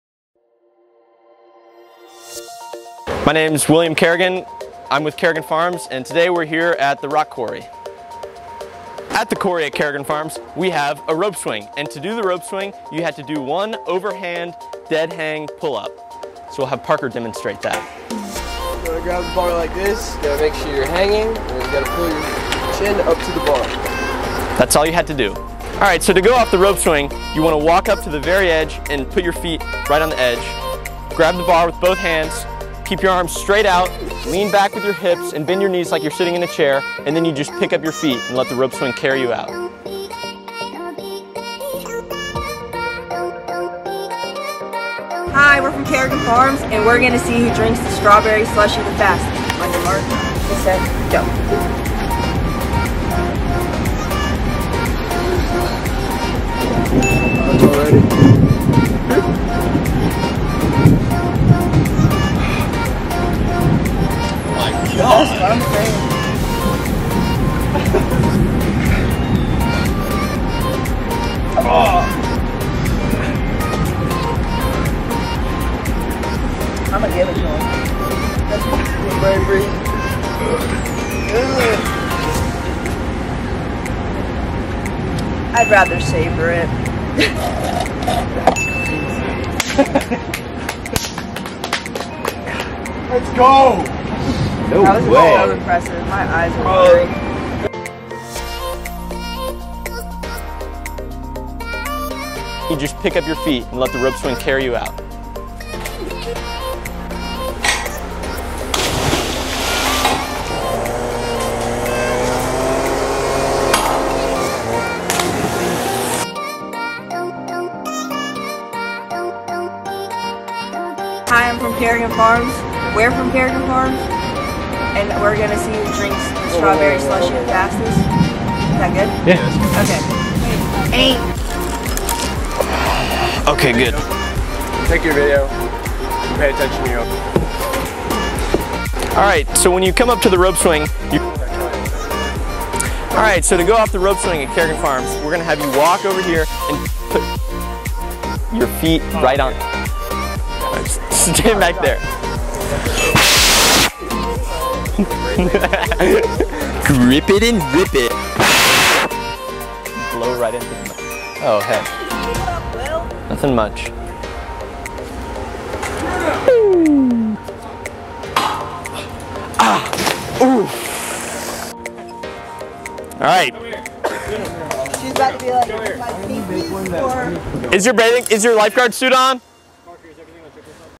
The Rope Swing with William
William talks about the Rope Swing at the Quarry and gives the instructions! (Also, bonus staff slushie drinking competition!)